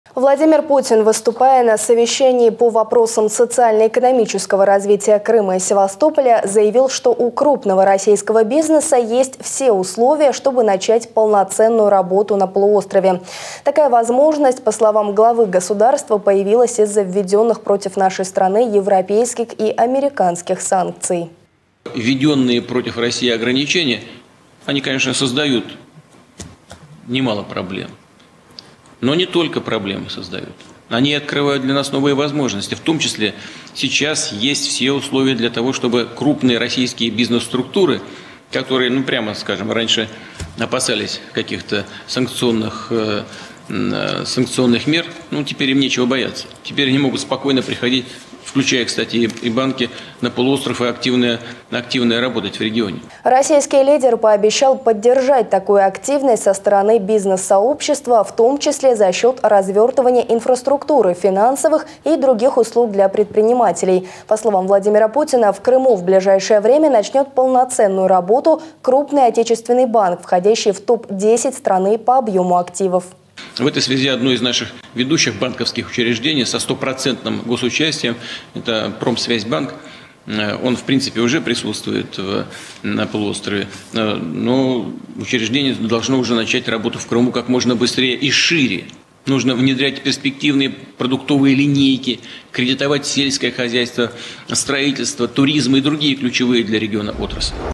Первый пошёл: Промсвязьбанк начнёт полноценную работу в Крыму и Севастополе
Владимир Путин, выступая на совещании по вопросам социально-экономического развития Крыма и Севастополя, заявил, что у крупного российского бизнеса есть все условия, чтобы начать полноценную работу на полуострове. Такая возможность, по словам главы государства, появилась из-за введённых против нашей страны европейских и американских санкций.